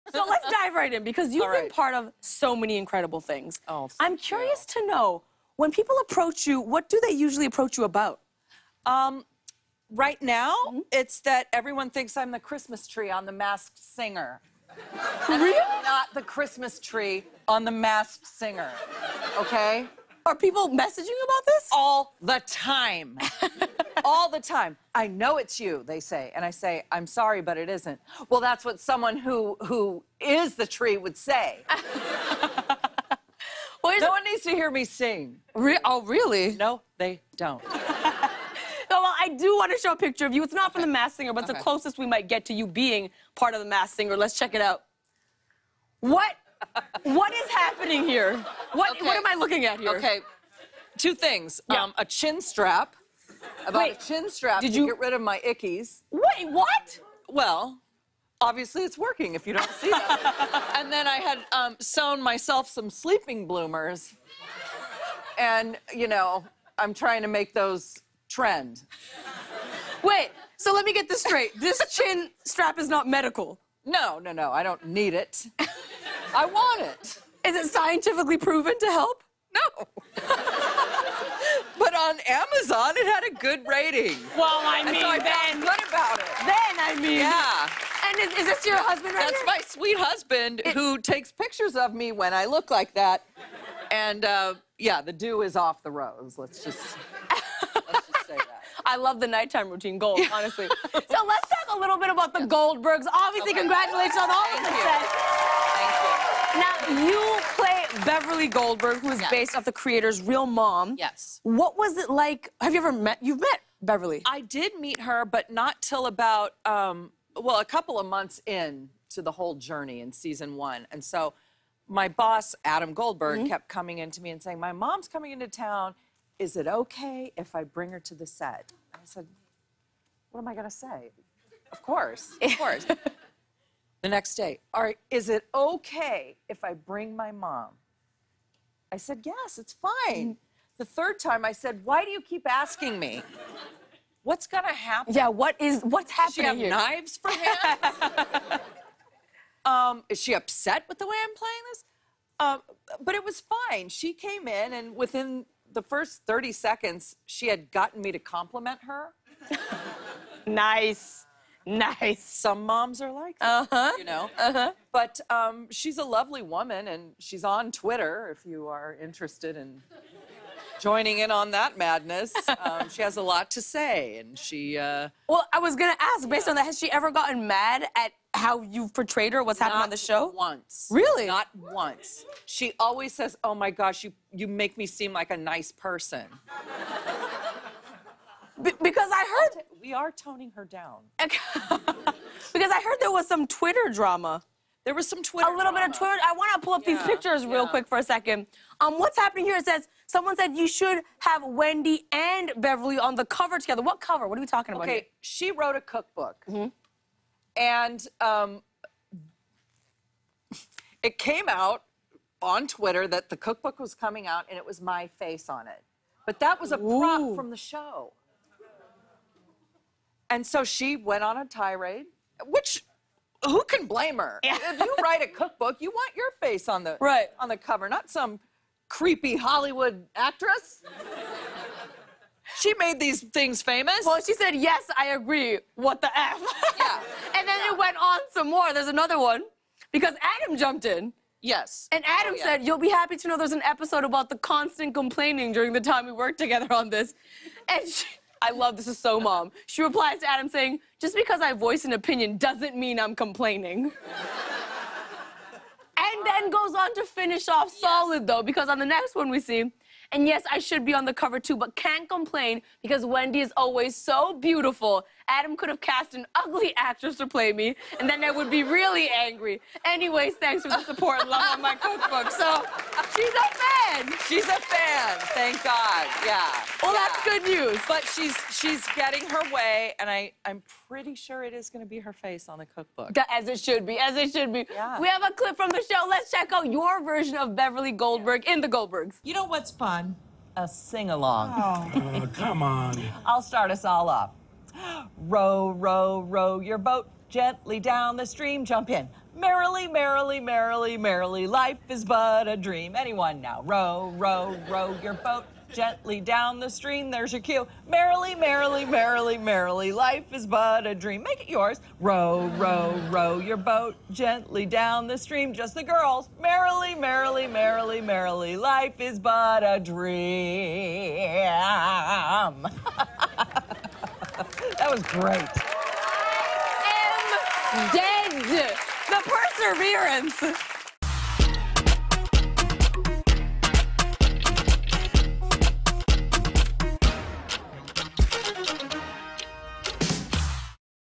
Wendi McLendon-Covey Addresses the Masked Singer Rumors
Wendi McLendon-Covey finally addresses those Masked Singer rumors and discusses her mother-in-law's opinion of how she is portrayed on The Goldberg's.